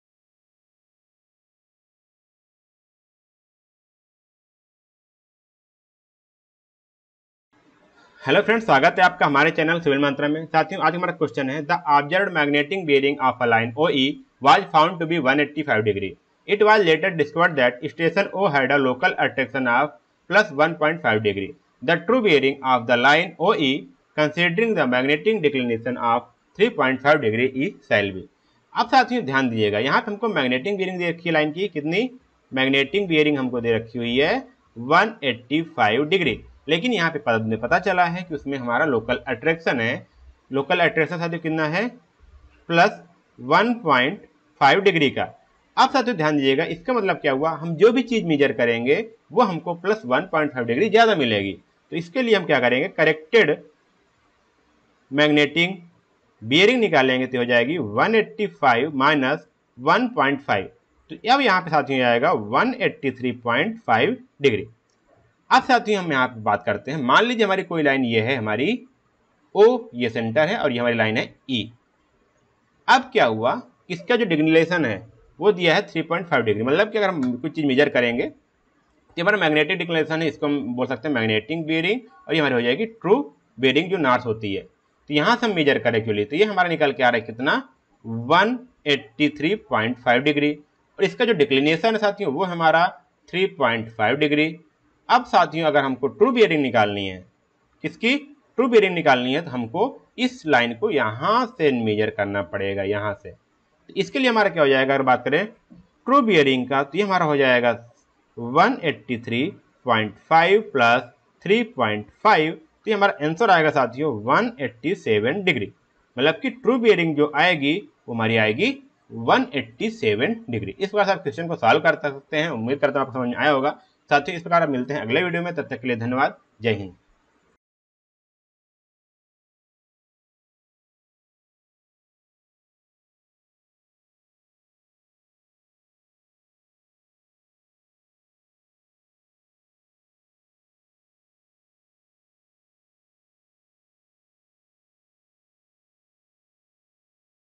The observed magnetic bearing of a line OE was found to be 185°. It was later discovered that statio
The observed magnetic bearing of a line OE was found to be 185°. It was later discovered that station O had a local attraction of +1.5°. The true bearing of the line OE, considering a magnetic declination of 3.5° E shall be